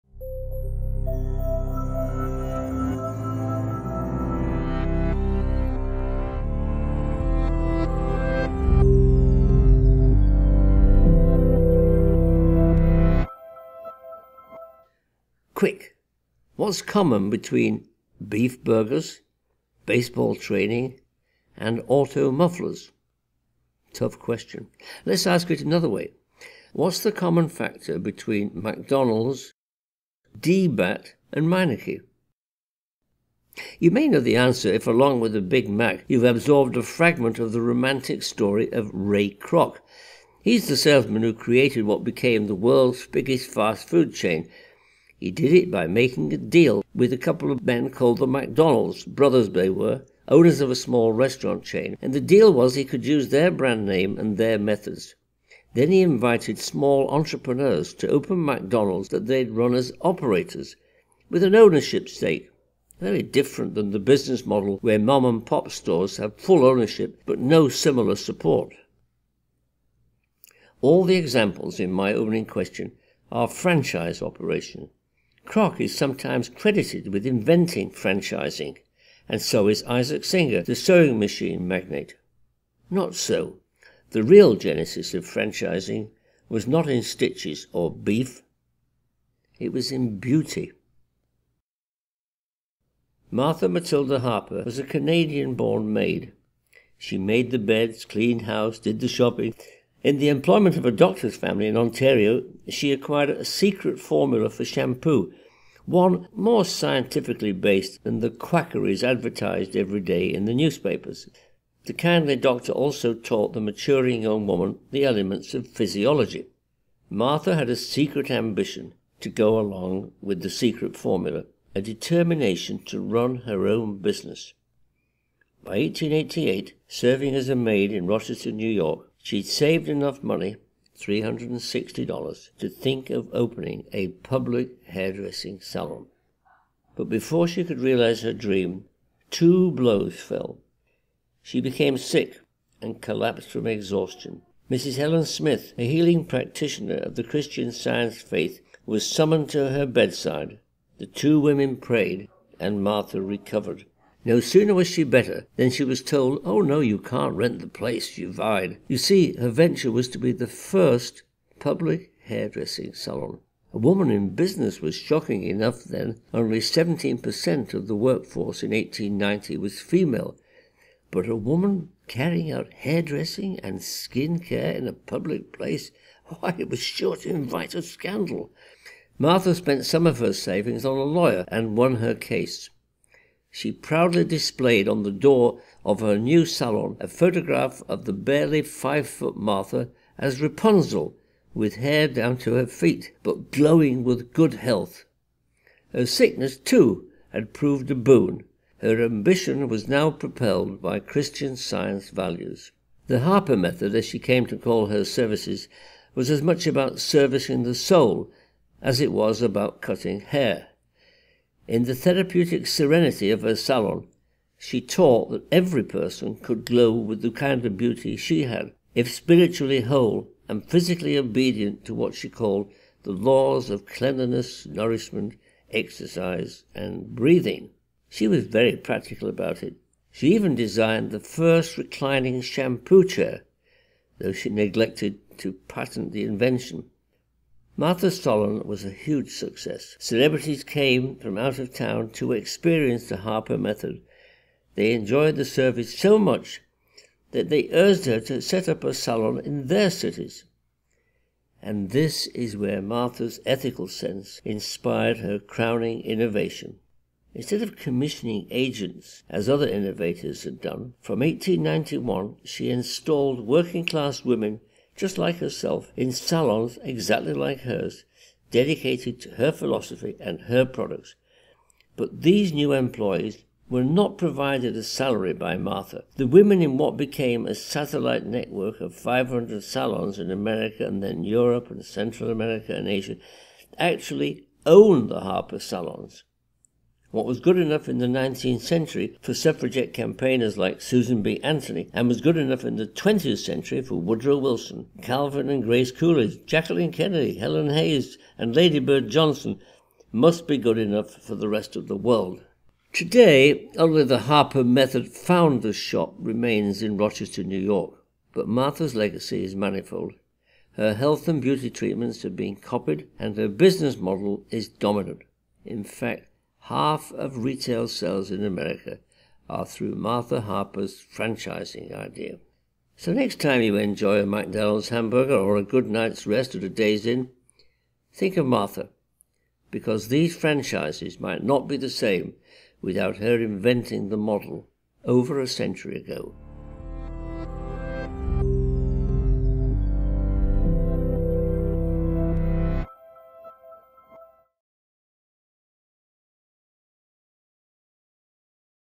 The Real Origin of the Franchise - Sir Harold Evans
One of the most successful business models is the franchise, but it didn't originate with McDonald's. Sir Harold Evans, the author of They Made America, describes the remarkable story of a beauty salon that allowed hundreds of women to own their own businesses.

Lesson by Sir Harold Evans. Animation by Sunni Brown.